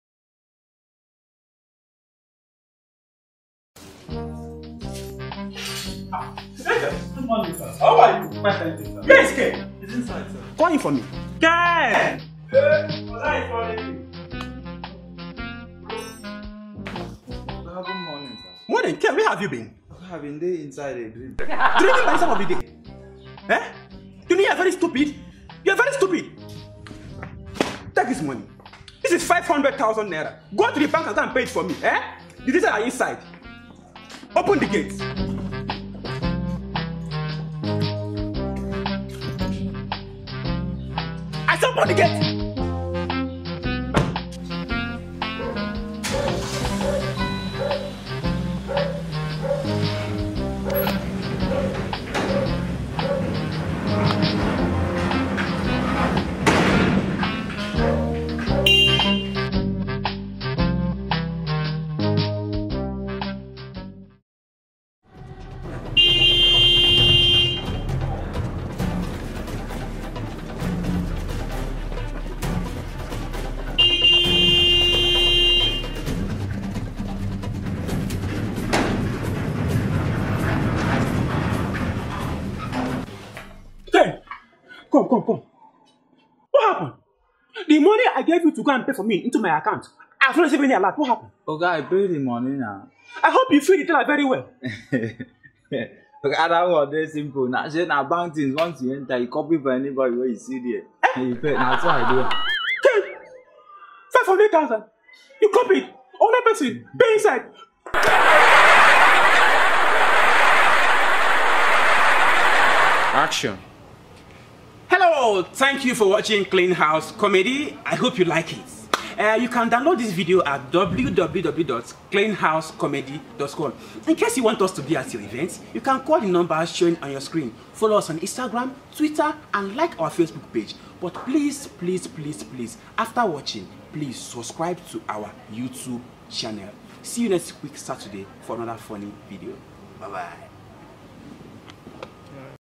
ALERT [Clean House Comedy] Episode 17
ken is at it again, Daddyhumble got the biggest shock of his life after sending ken to pay in money to his bank account. watch and see what ken did in the banking hall.